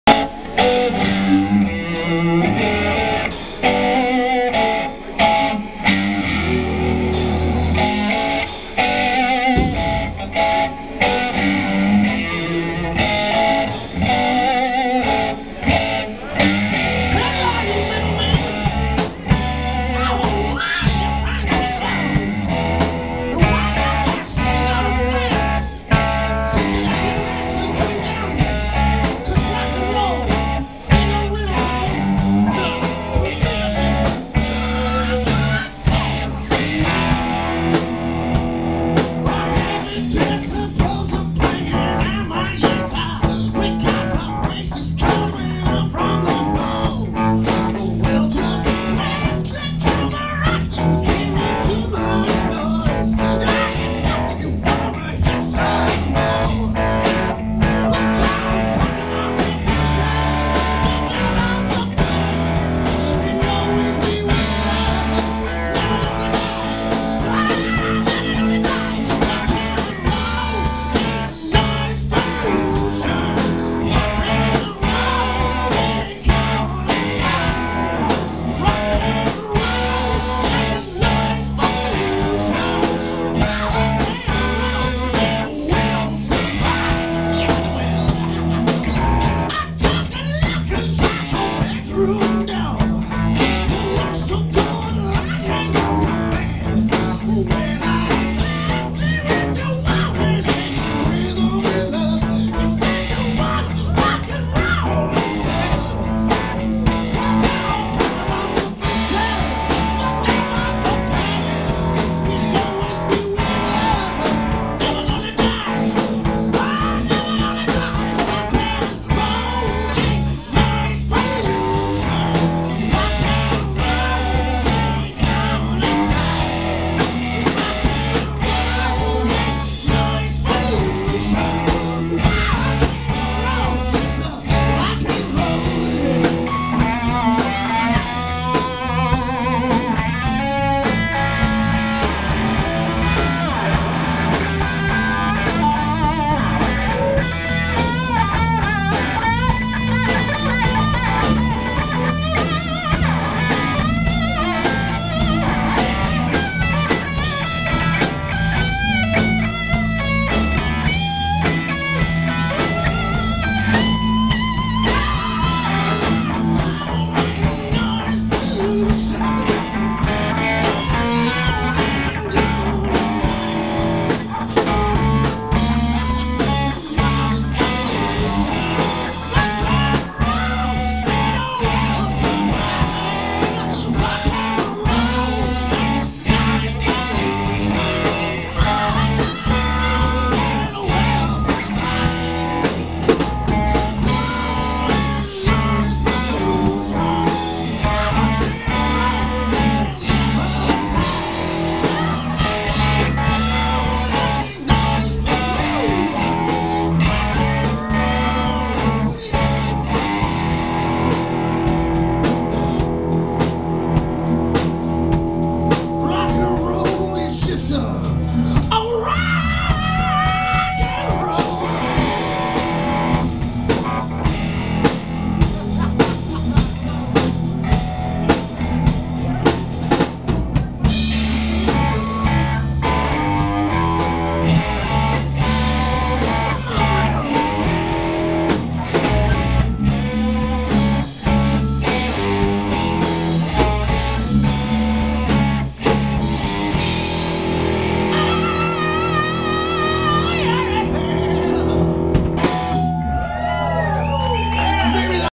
Renegades Sunday Open Jam - Tim
Sunday Open Jam - Tim Swanson on vocals. Corey Bellucci on guitar. Larry Lemery on guitar and vocals. Ed on drums. Joe Coccia on bass. Open jam every Sunday at Renegades, Warwick RI